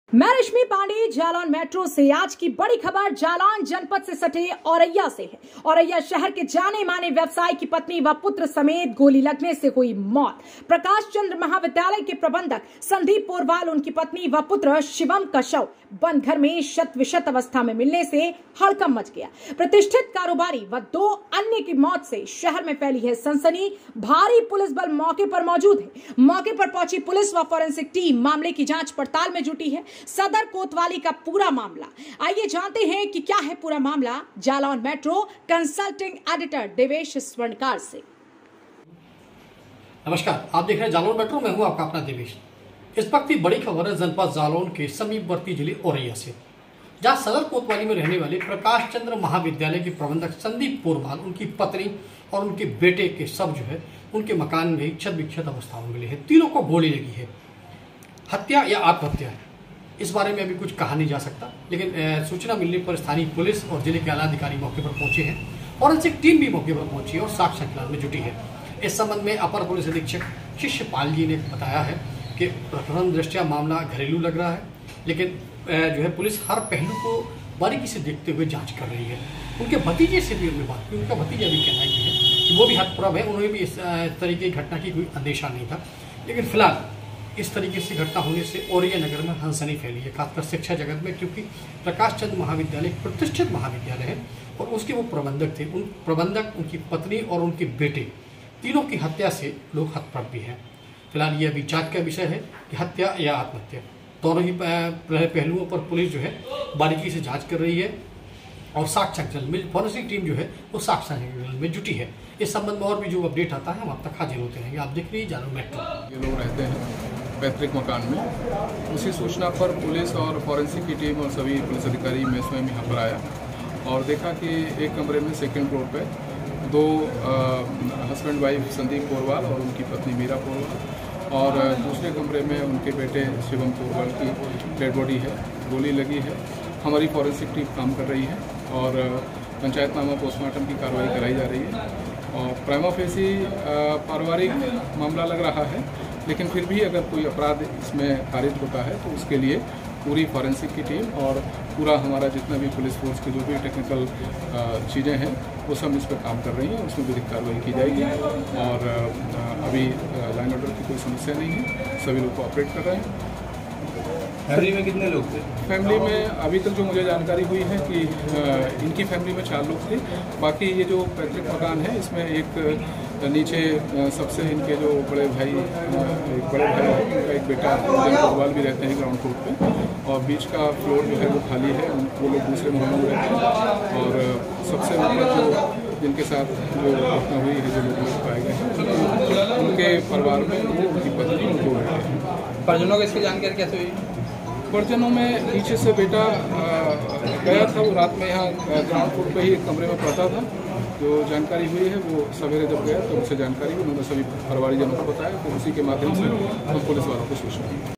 जालौन जनपद से लगे औरय्या में प्रतिष्ठित कारोबारी व 2 अन्य की मौत से शहर में फैली सनसनी देखिए रिपोर्ट
आज की बड़ी खबर जालौन जनपद से सटे औरय्या से है

औरय्या शहर के जाने माने व्यवसायी की पत्नी व पुत्र समेत गोली लगने से हुई मौत

प्रकाशचंद्र महाविद्यालय के प्रबंधक संदीप पोरवाल उनकी पत्नी व पुत्र शिवम का शव बन्द घर में क्षत विक्षत अवस्था मे मिलने से मचा हड़कंप

प्रतिष्ठित कारोबारी व 2 अन्य की मौत से शहर में फैली सनसनी

भारी पुलिसबल मौके पर मौजूद

मौके पर पहुंची पुलिस व फोरेंसिक टीम मामले की जांच में जुटी

जालौन मैट्रो कंसल्टिंग एडिटर देवेश स्वर्णकार की रिपोर्ट.